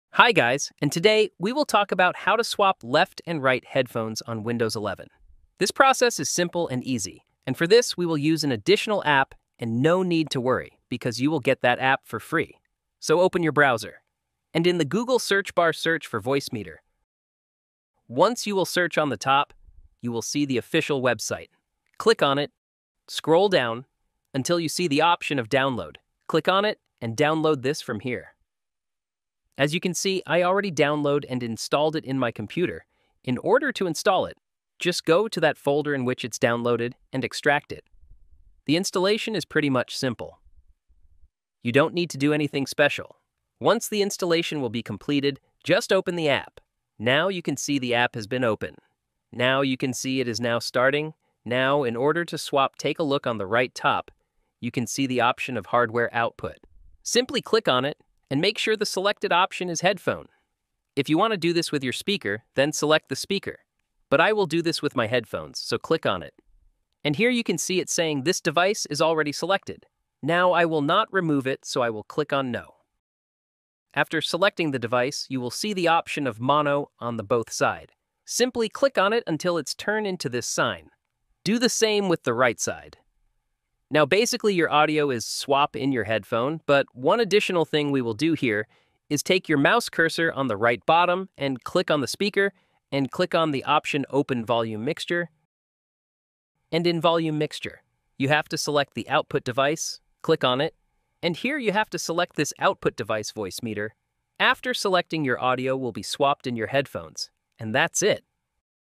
How To Swap Left & Right Headphones in Windows 11 ✅
Learn how to swap headphone sides in Windows 11 with ease! Are you tired of dealing with the hassle of switching headphone sides every time you want to listen to music or watch videos? In this tutorial, we'll show you a simple trick to swap headphone sides in Windows 11, so you can enjoy an uninterrupted audio experience. Whether you're a gamer, music lover, or simply someone who likes to relax with their favorite shows, this tip is a game-changer. So, what are you waiting for? Watch now and discover how to swap headphone sides in Windows 11 easily!